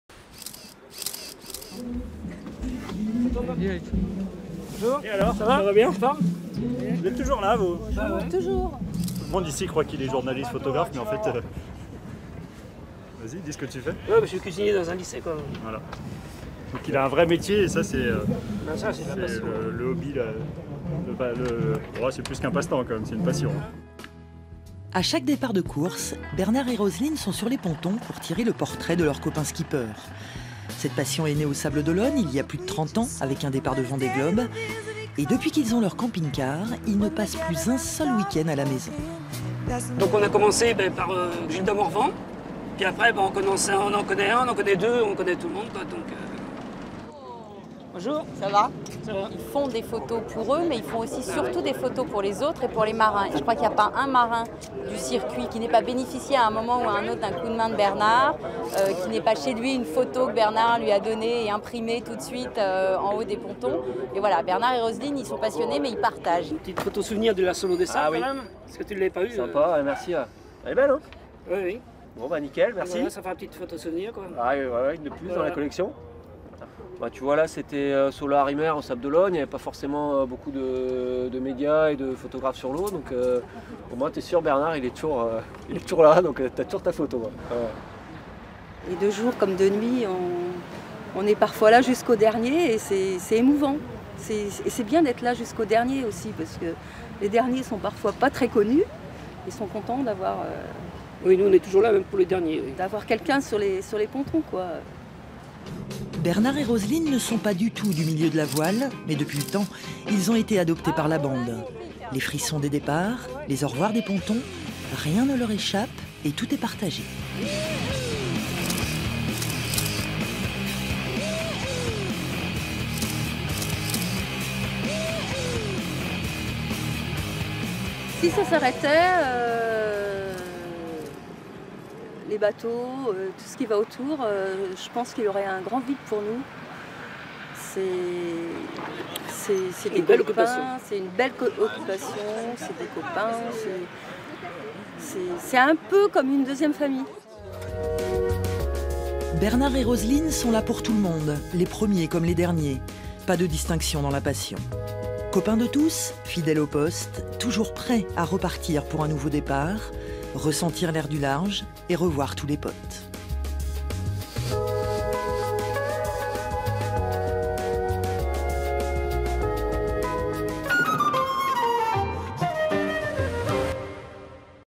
Sur la route des courses
Bernard et Roselyne font partie de cette humanité vraiment généreuse. Ca fait plus de trente ans qu’ils prennent des photos de tous les navigateurs, célèbres ou pas, et qu’ils distribuent leurs clichés comme des cadeaux. Copains de tous, et toujours fidèles.